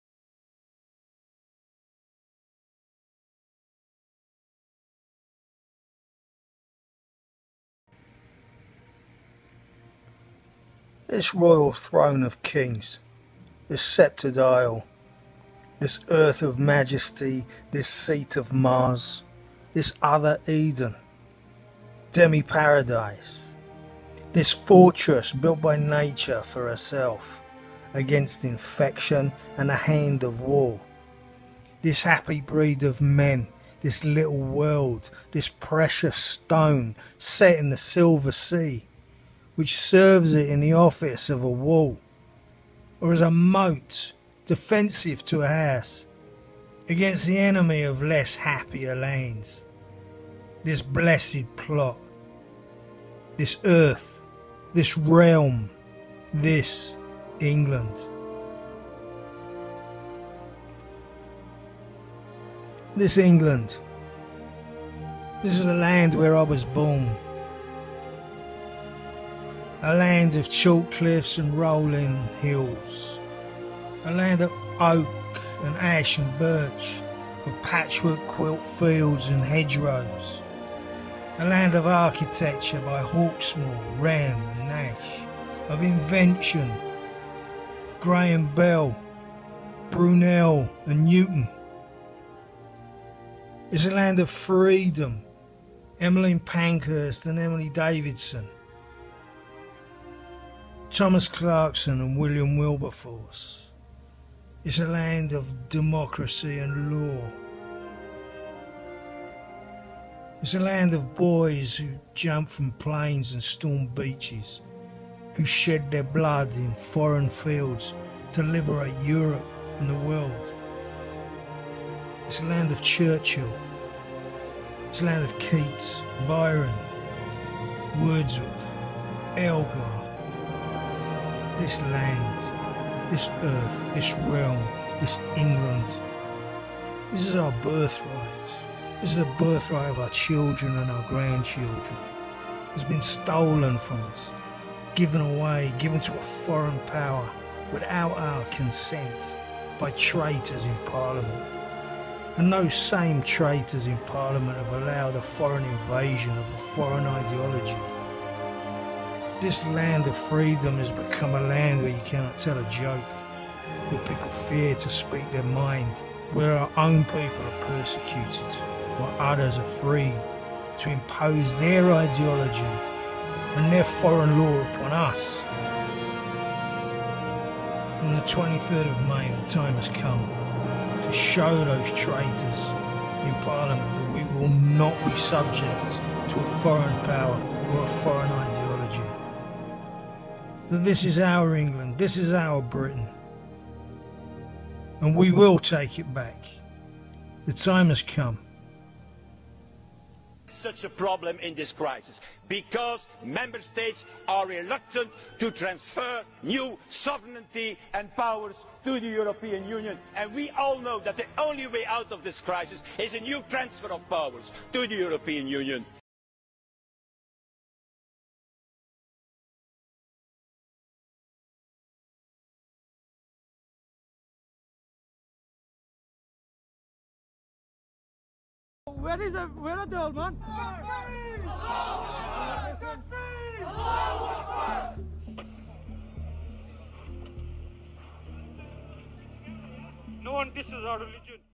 This Sceptred Isle : Your Vote Matters
Three years ago Britain voted to leave the European Union. Traitors and insidious fifth column operators within both houses of Parliament and the British media have deliberately sabotaged the leaving process in an attempt to overturn the UK's EU Referendum result.

We now find ourselves voting for MEPs at a time we should have already left. The EU bureaucrats have demonstrated time and time again they intend to subjugate Britain with the aid of those treacherous MPs who ignore the people of Britain.

At the same time, an Islamic uprising is taking place, imposing their own ideology upon the British people, turning the cowardly authorities against their own citizens.

It is time to take back our country.
23rd May in the European elections Vote UKIP, Vote Brexit Party, Vote Tommy

Send a clear message that Britains will never be slaves and this Sceptered Isle is ours.